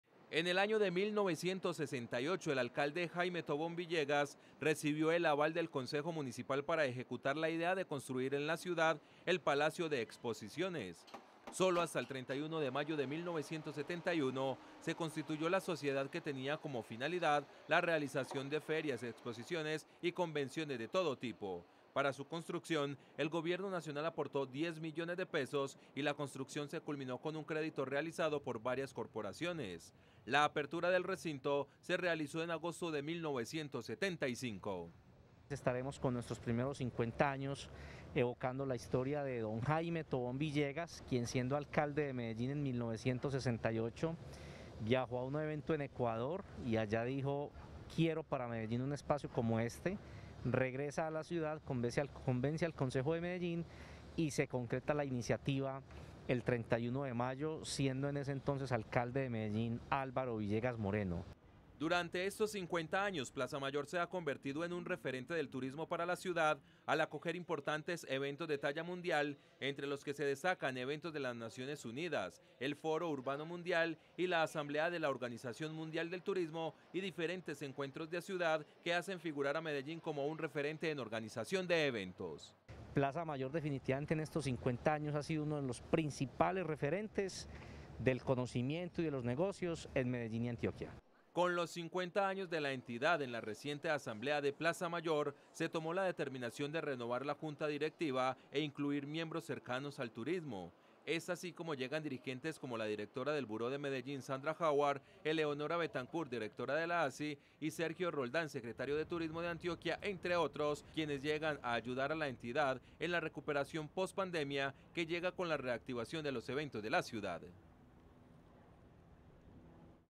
Plaza Mayor cumple 50 años desde su fundación - Telemedellín
Este 31 de mayo uno de los recintos más emblemáticos de la   está de celebración.  Plaza Mayor cumple 50 años de fundación, tiempo en el que se han realizado los eventos más importantes convirtiendo a Medellín en un referente de organización de eventos.